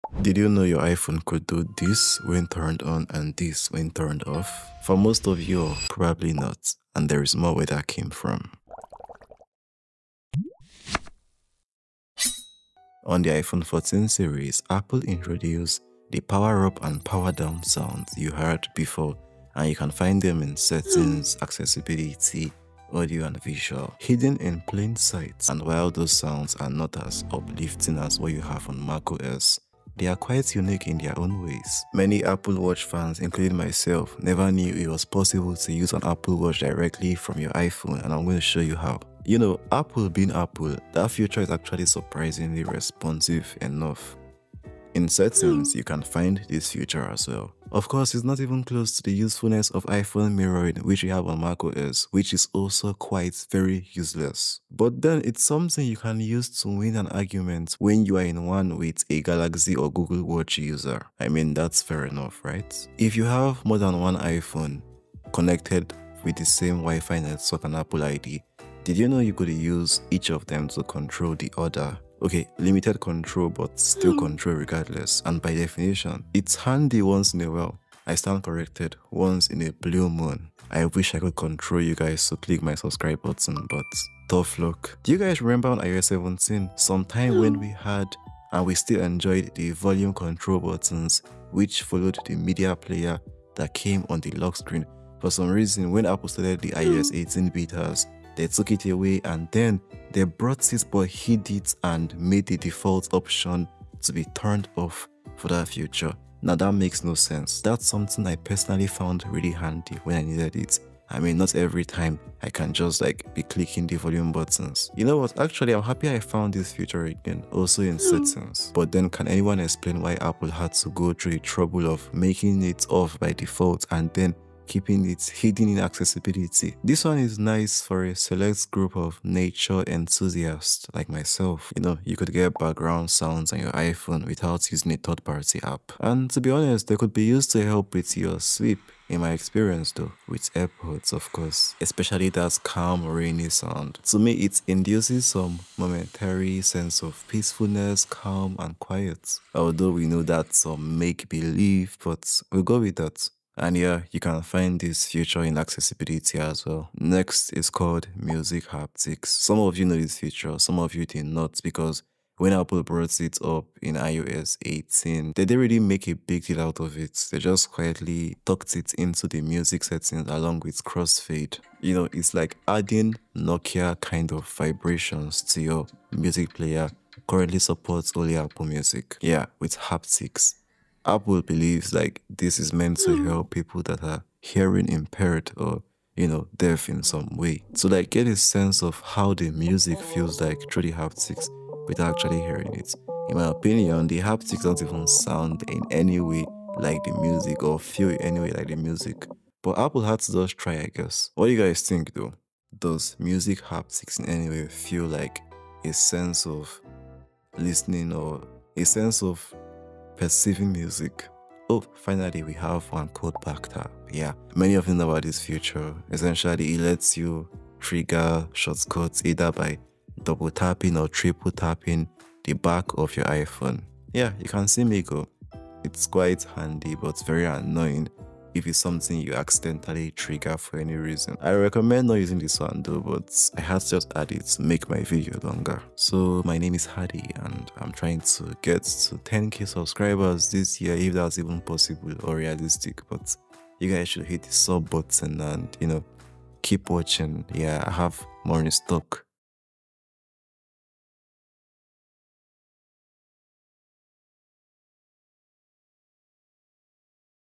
The Secret iOS Features You Didn't Know About
Some cool, maybe the best iOS features hidden in plain sight in your iPhone. Some of these might not be supported on all iPhone models.
CHAPTERS
0:00 feature 1
0:32 feature 2
1:02 feature 3
1:24 feature 4
2:05 feature 5
2:36 feature 6
3:31 feature 7